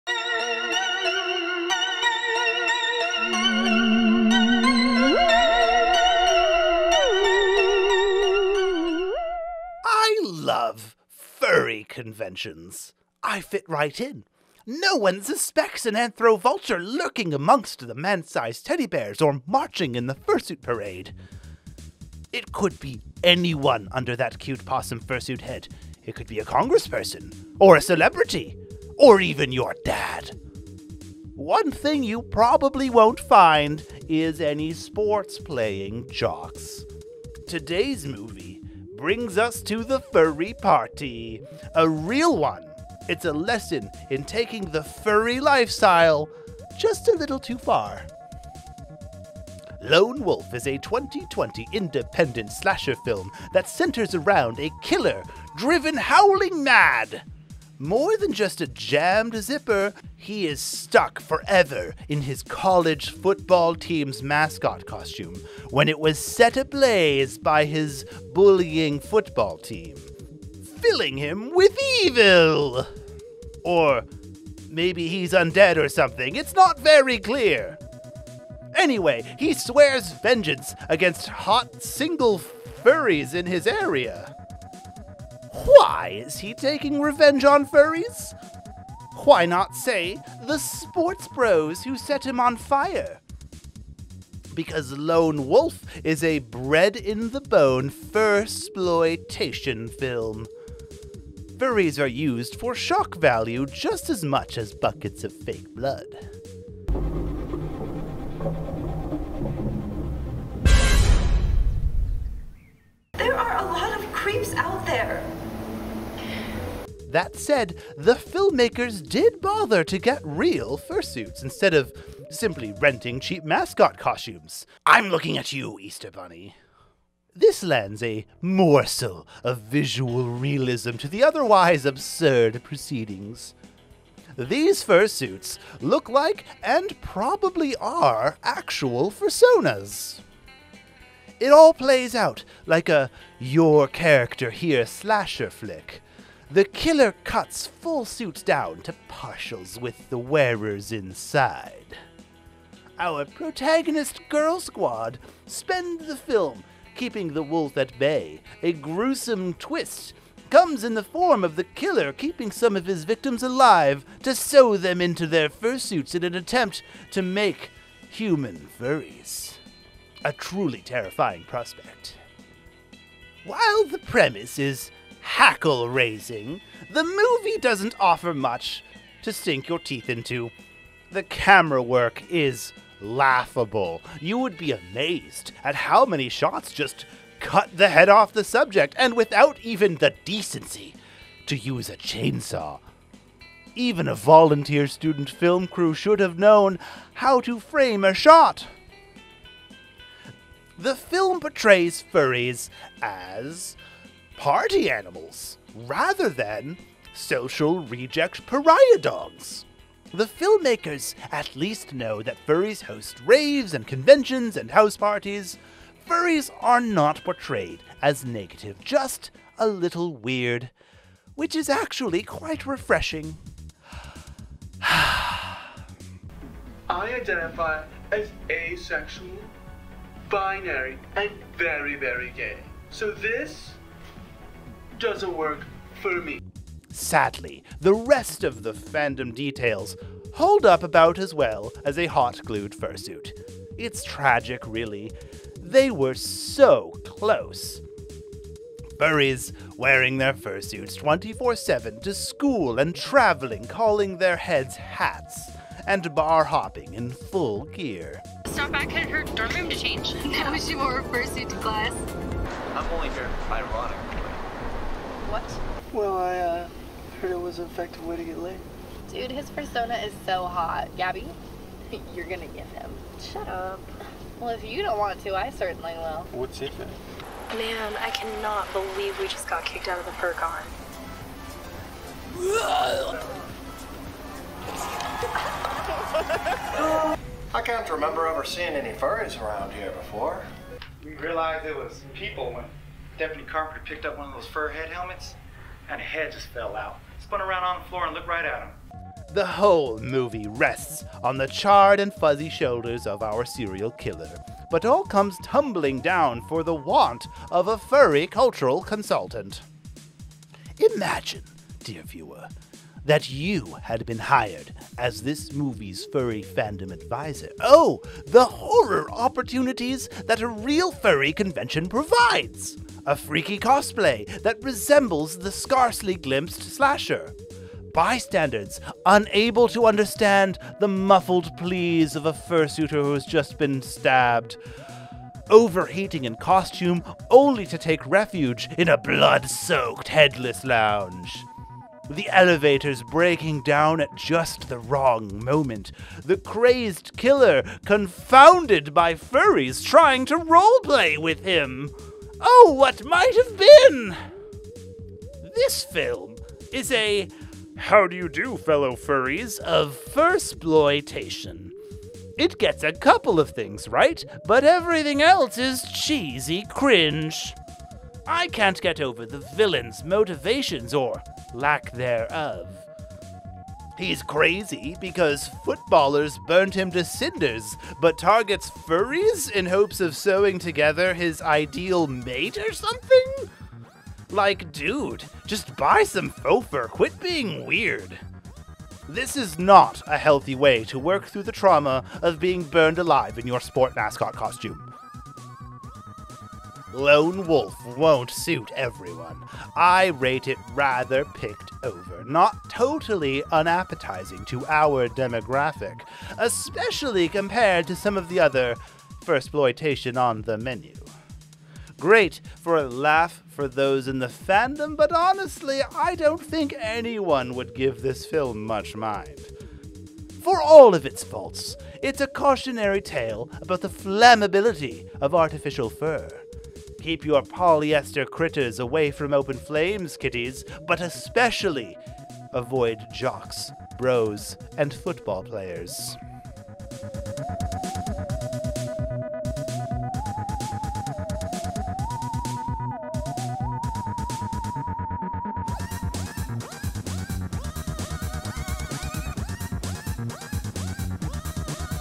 Lone Wolf (2020) | Bone Zone B Movie Reviews
Waiter, there's FUR in my meal~ Today Rattles savours the flavours of this fursploitation indie film featuring furries, "Lone Wolf" (2020)